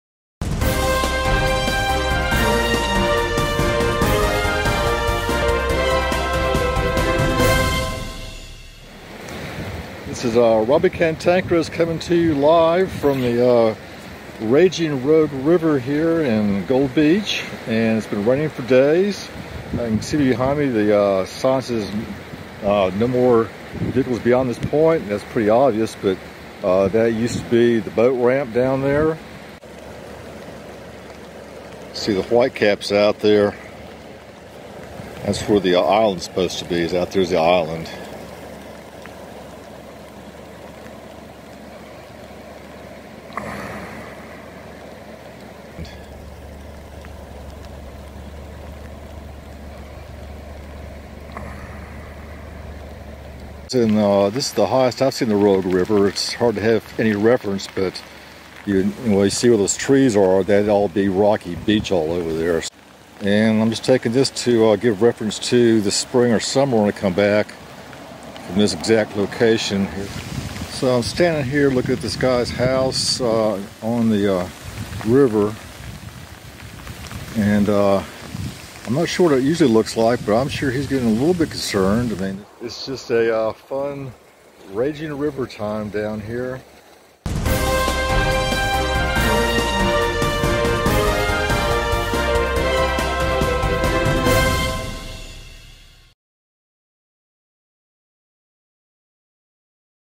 Rogue River Gold Beach Weather report with Robbie Cantankerous
Producer/Director/Post - Robert Lagerstrom.  Private promotional use only.